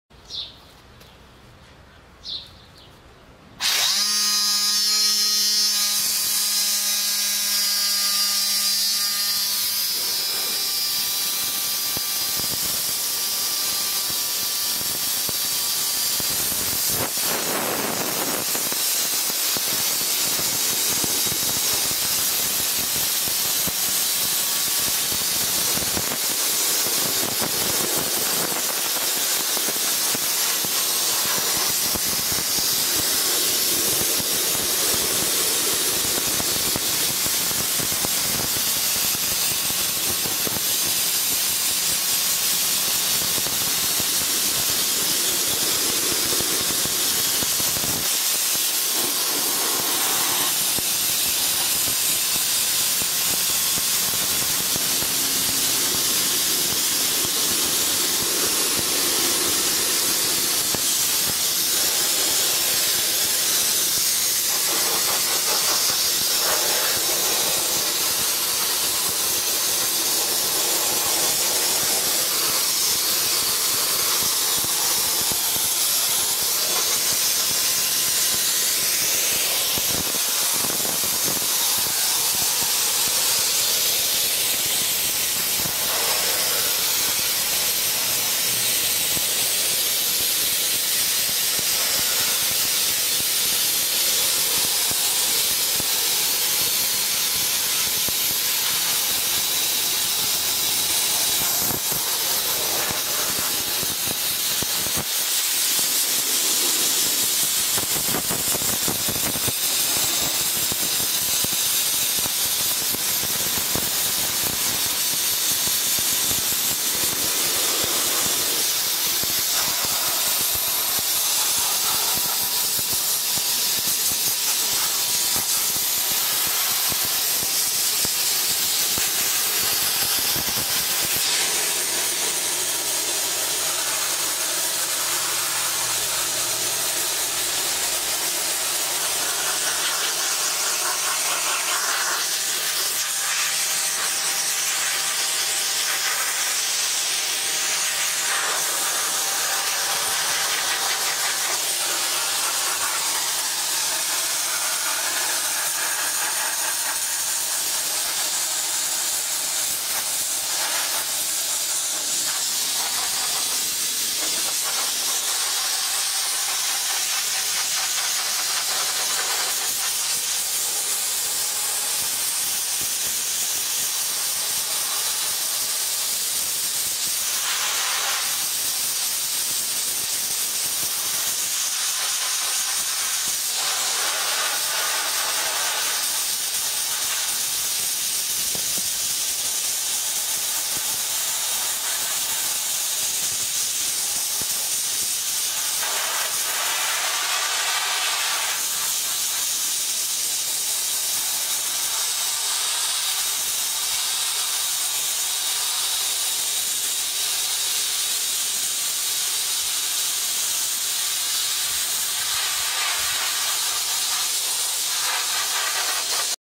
Sodablaster TESI 40 Eco Line
Peskanje s sodo ima blag učinek in popolnoma nič ne poškoduje obdelovalno površino. Še posebej pomebno je pri peskanju, kjer so prisotne gredi in osi motorjev, saj lahko po končanem peskanju enostavno odstranimo abraziv z zrakom ali vodo.